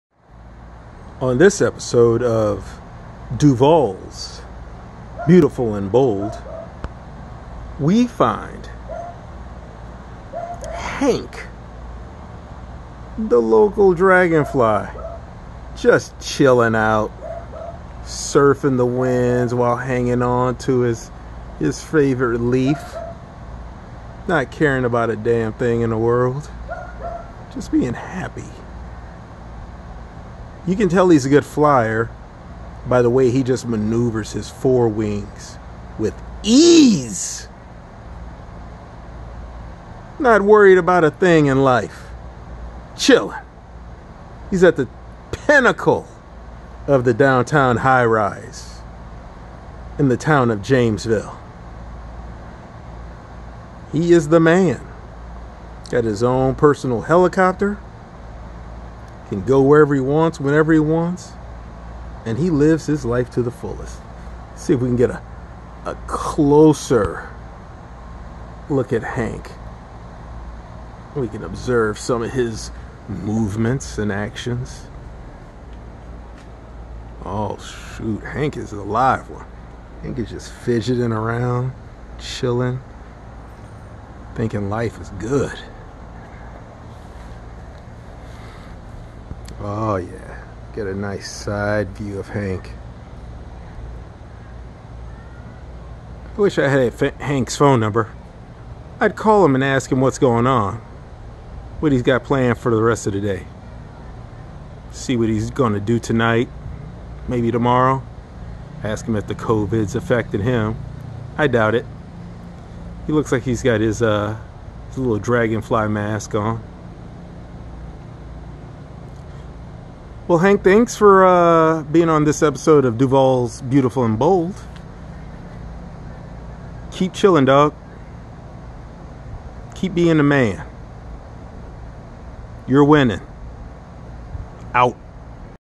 Dragonfly Hank shows off his chopper.
Hank shows off his landing skills.